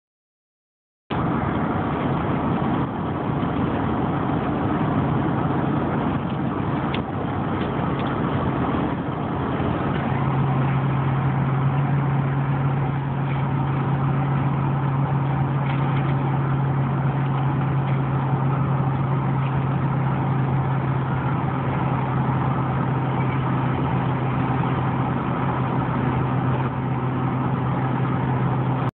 2012 Audi S8 with a v10
a nice black audi s8 on the highway with power and smooth also. im a big fan of audi's